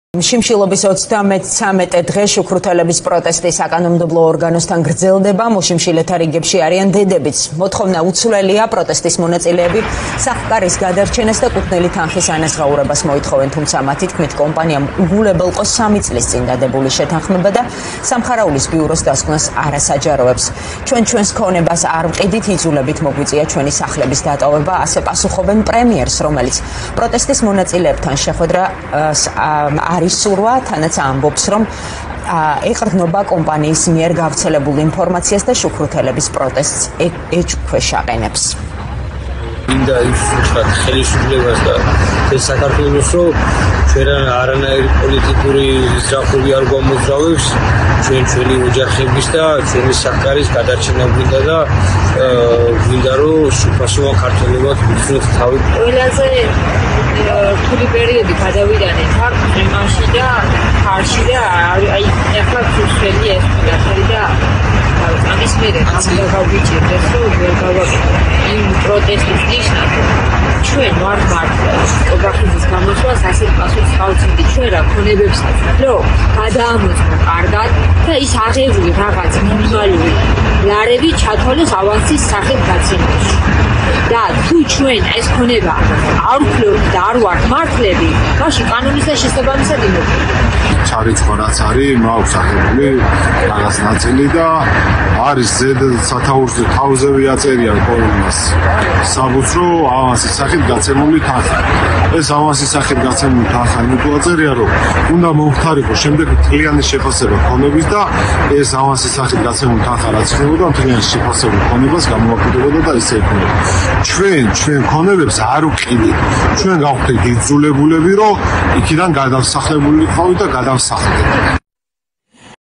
მოშიმშილე შუქრუთელები პარლამენტის წინ რჩებიან
▪️  „პოლიტიკური ზრახვები არ გვამოძრავებს, ჩვენ ჩვენი ოჯახებისა და სახლ-კარის გადარჩენა გვინდა“ — შუქრუთელების პროტესტი პარლამენტთან გრძელდება. ისინი უკვე 33-ე დღეა შიმშილობენ